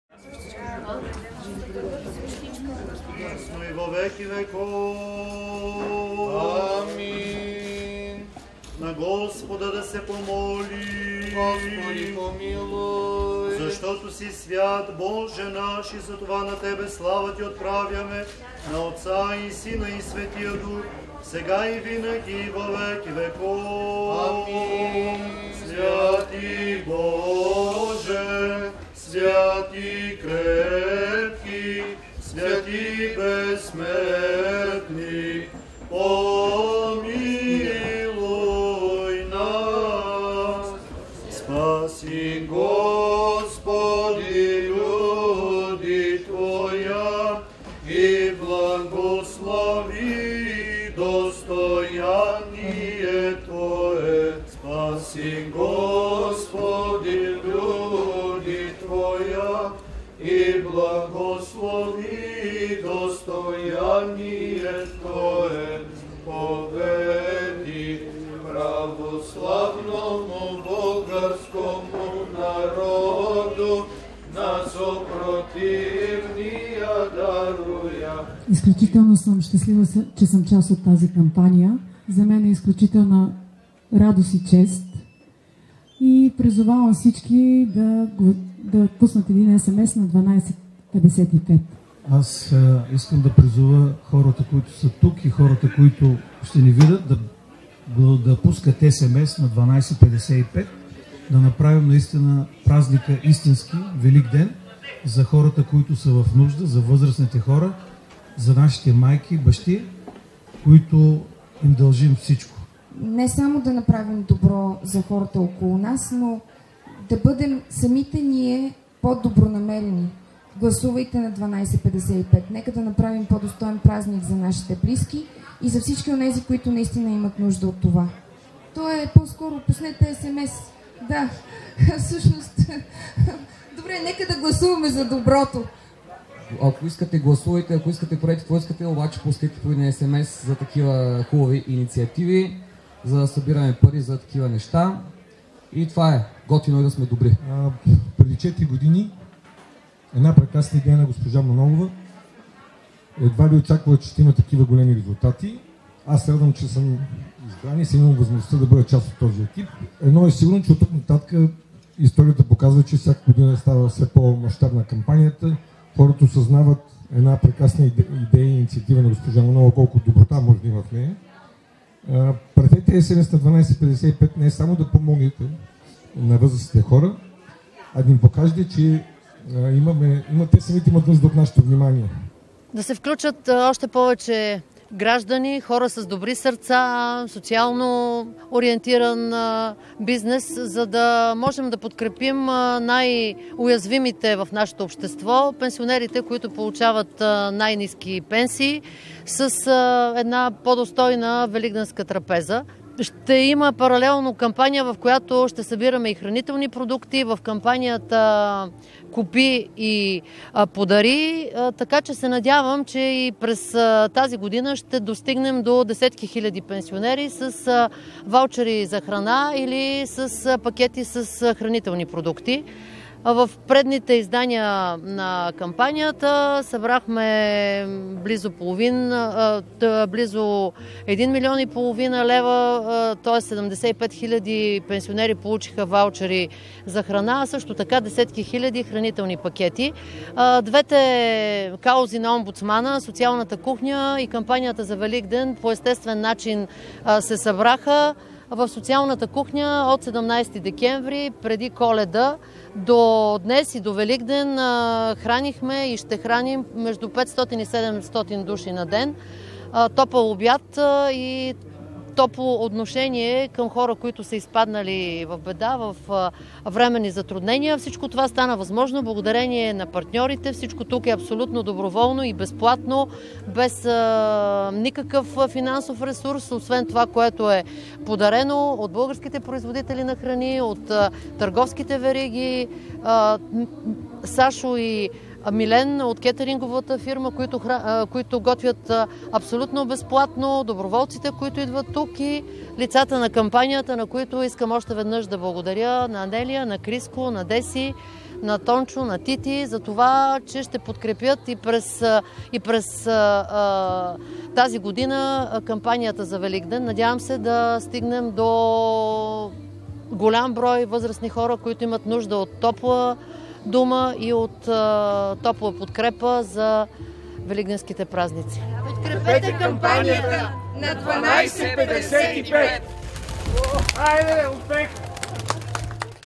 Великден за всеки – дари празник на баба и дядо
На Благовещение с молебен за здраве и благословение, омбудсманът Мая Манолова обяви началото на четвъртото издание на Националната кампания „Великден за всеки – дари празник на баба и дядо“. /БГНЕС